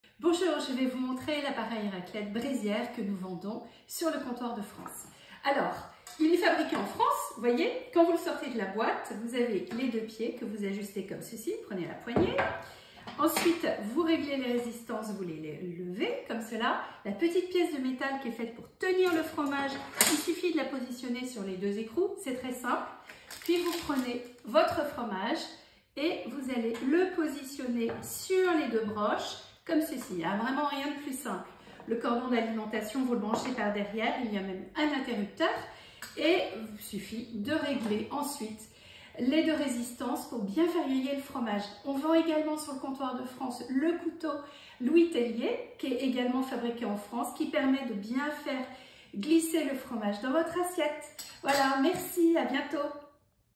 Appareil à raclette Brézière Louis Tellier
Découvrez comment installer l'appareil à raclette Brézière de Louis Tellier. Idéal pour la raclette traditionnelle, l'appareil à raclette à Brézière Louis Tellier est en vente sur Le Comptoir de France.